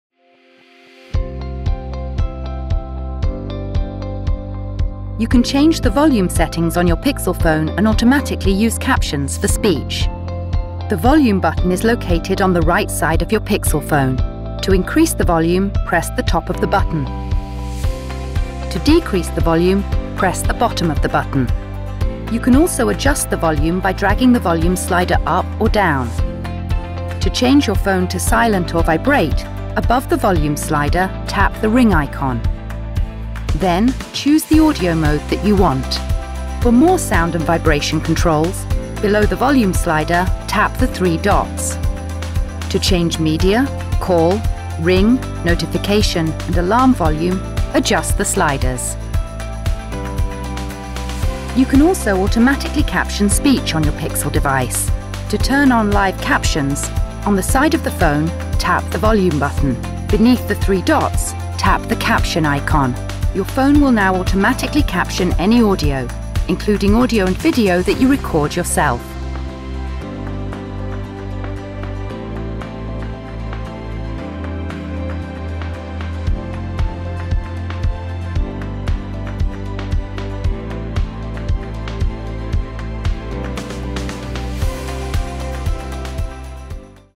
Change your volume settings on your Pixel phone
Learn how to change your volume settings, including silence or vibrate on your Pixel phone.

Some features may not work on all Pixel devices.

To learn more about how to change your volume settings on your Pixel phone